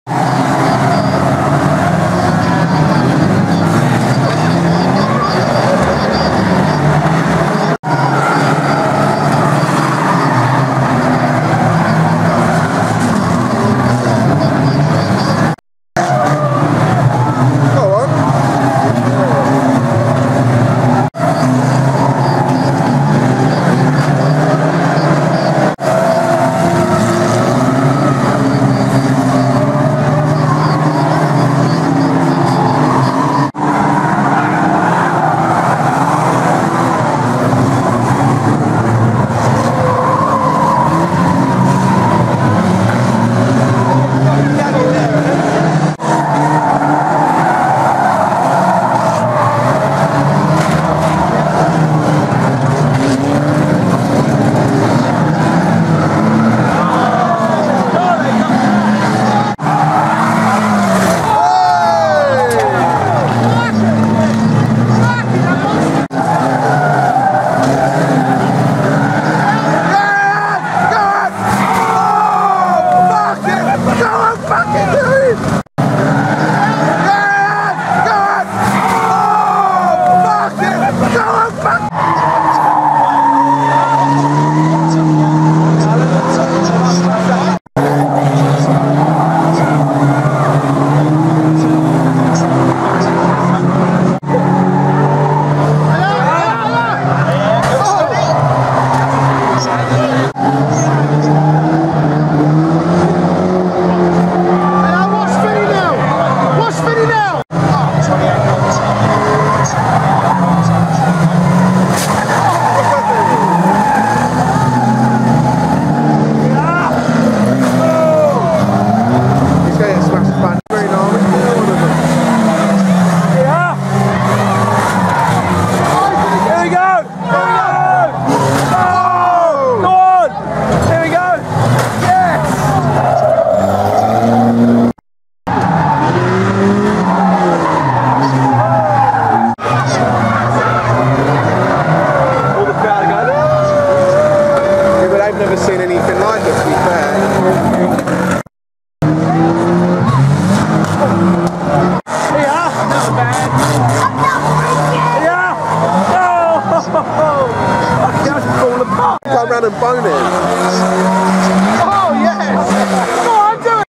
Skegness Crash Attack 2012
Just over 20 cars turned up for Skegness annual Crash Attack. This video features some highlights from the meeting